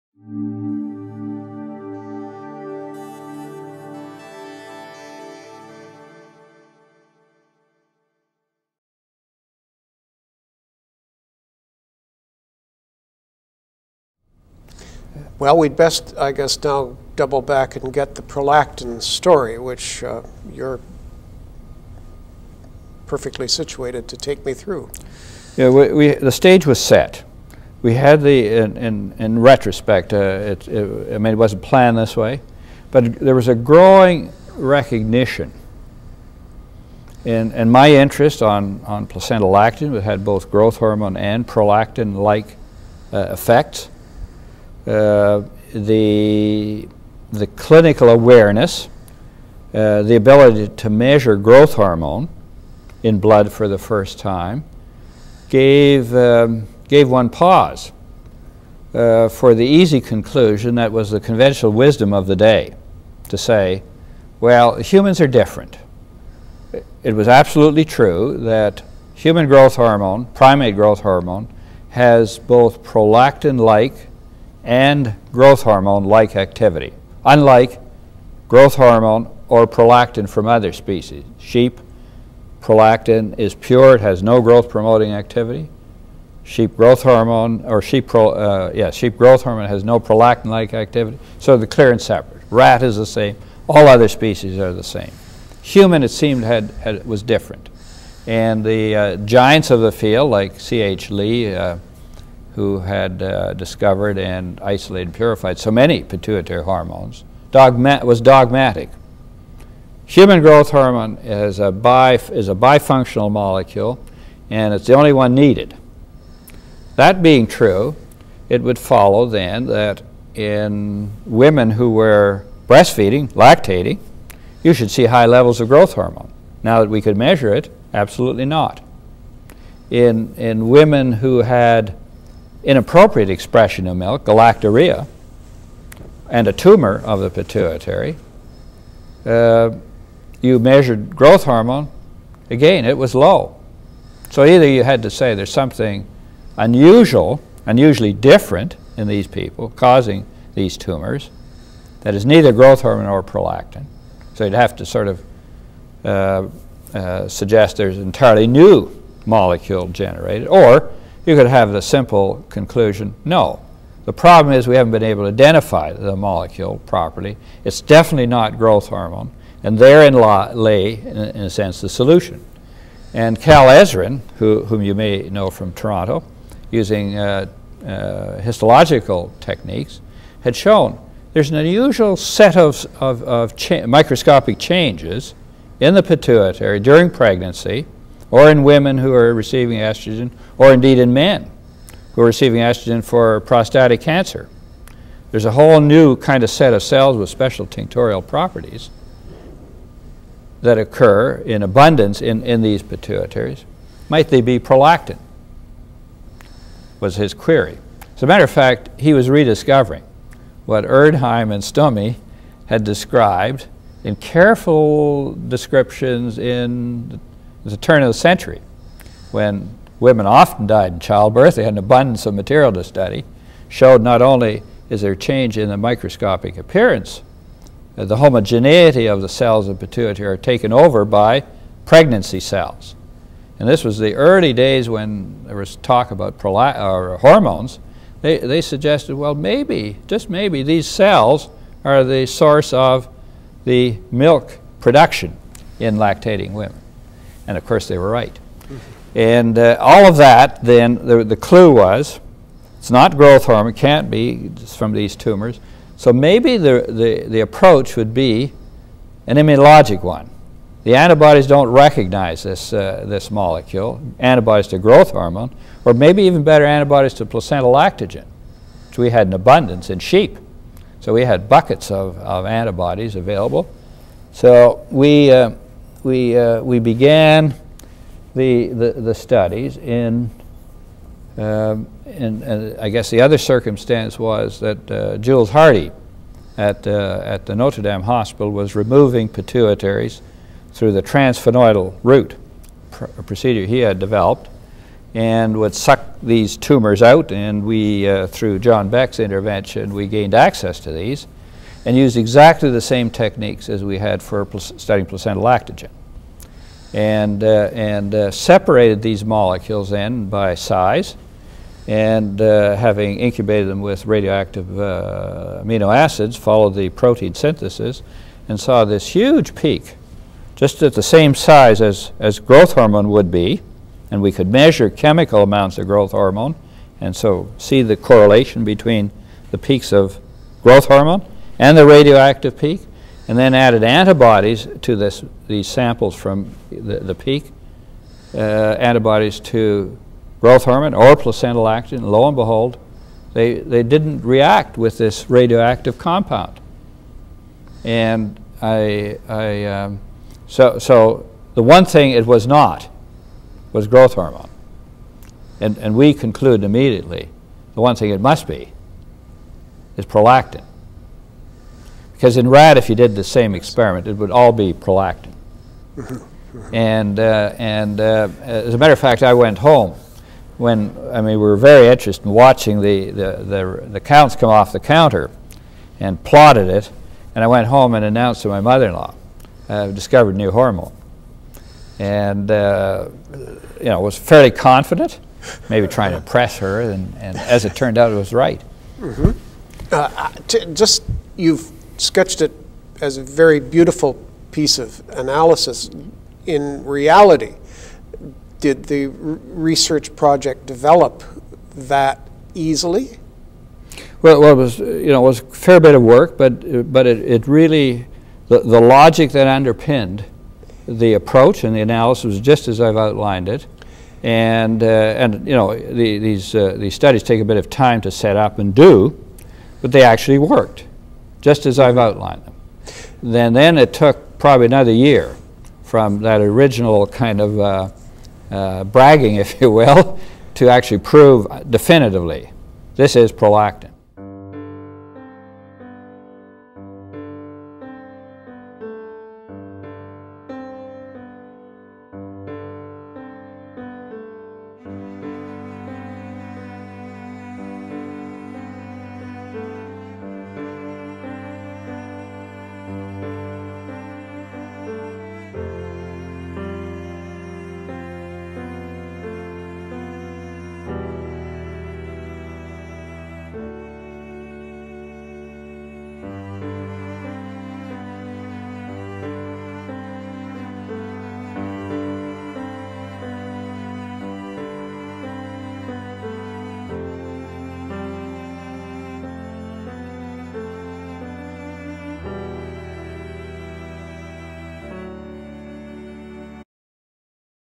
Dr. Henry G. Friesen: The Prolactin Story
Excerpt From the Video History of Medicine in Canada Project of the FCIHR

An excerpt from Prof. Michael Bliss'  interview of Dr. Henry Friesen.